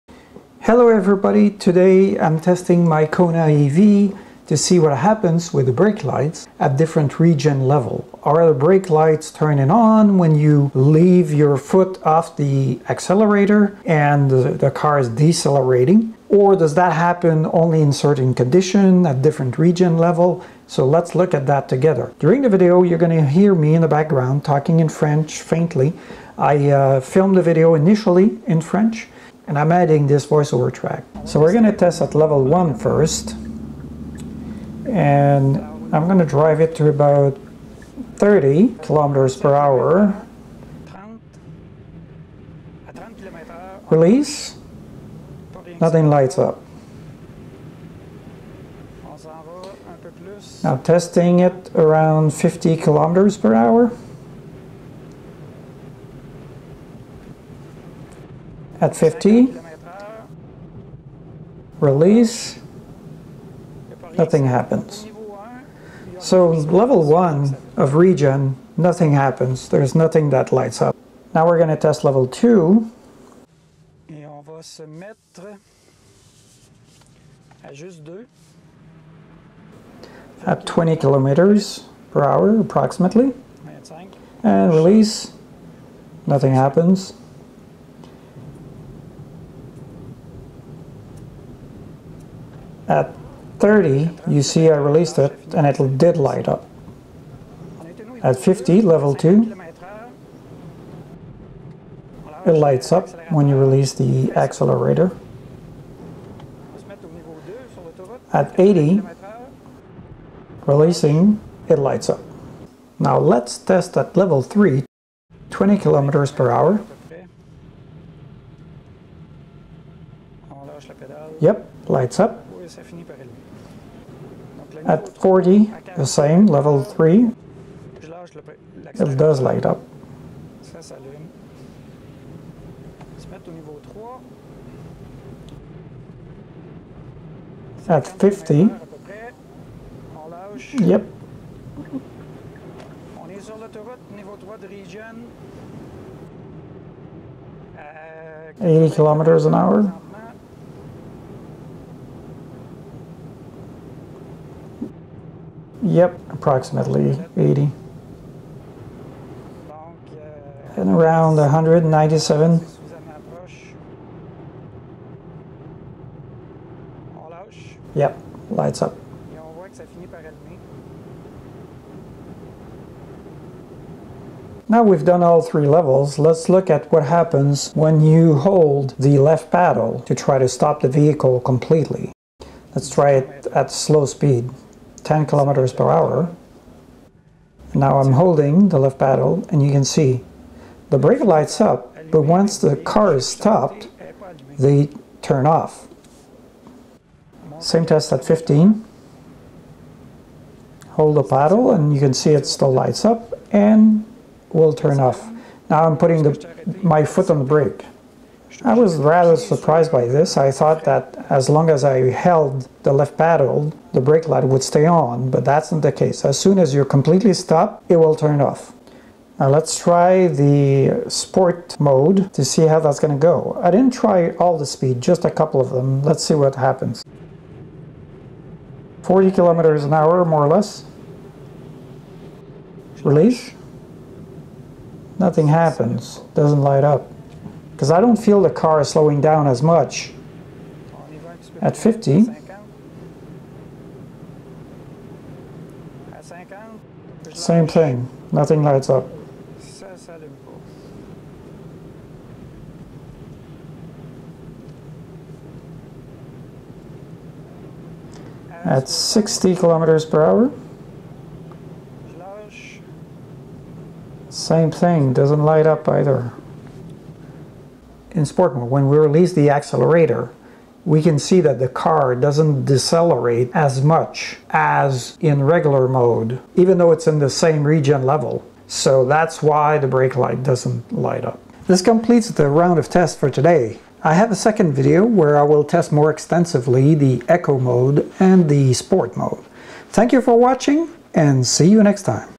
Kona EV 2019 - Regen versus brake lights
When using regen to slow down the Kona EV, will the brake lights turn on?